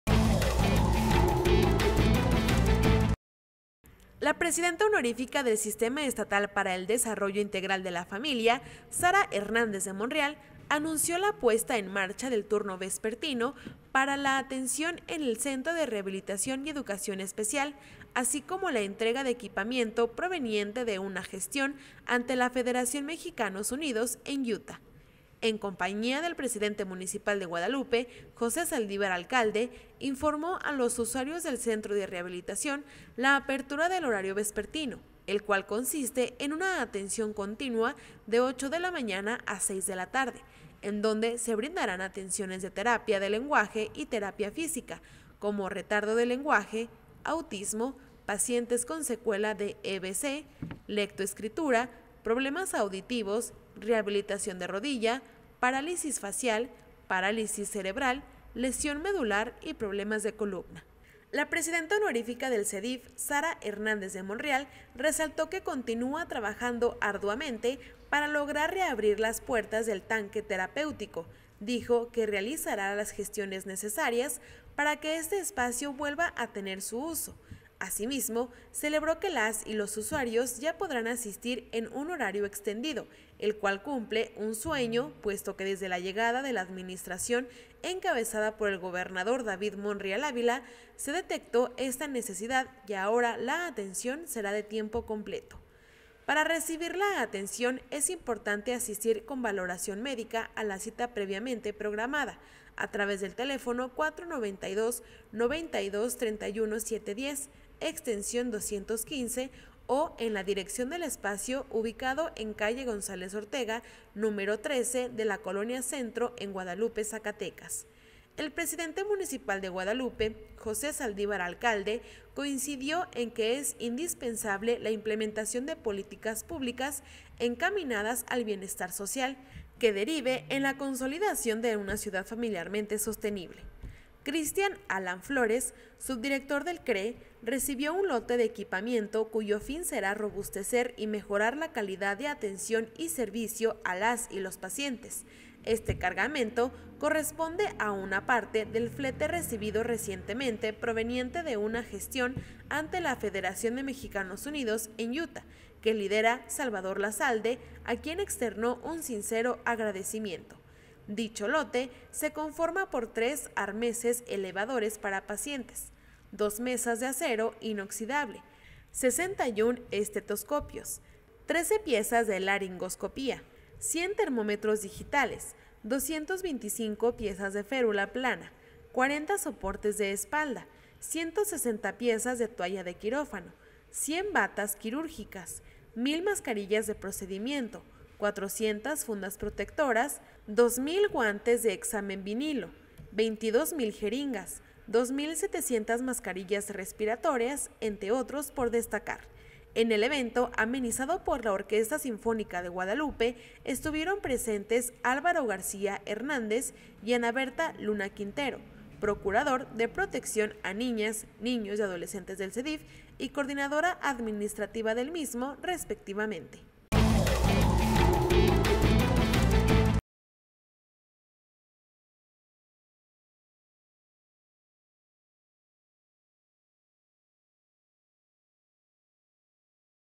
Anuncia Presidenta del SEDIF extensión de horario en el CREE y entrega lote de equipamiento
La Presidenta Honorífica del Sistema Estatal para el Desarrollo Integral de la Familia (SEDIF), Sara Hernández de Monreal, anunció la puesta en marcha del turno vespertino para la atención en el Centro de Rehabilitación y Educación Especial (CREE), así como la entrega de
equipamiento proveniente de una gestión ante la Federación de Mexicanos Unidos en Utah.
En compañía del Presidente Municipal de Guadalupe, José Saldivar Alcalde, informó a los usuarios del Centro de Rehabilitación, la apertura del horario vespertino, el cual consiste en una atención continua de 08:00 18:00 horas, en donde se brindarán atenciones de terapia de lenguaje y terapia física como: retardo de lenguaje, autismo, pacientes con secuela de EVC, lectoescritura, problemas auditivos, rehabilitación de rodilla, parálisis facial, parálisis cerebral, lesión medular y problemas de columna.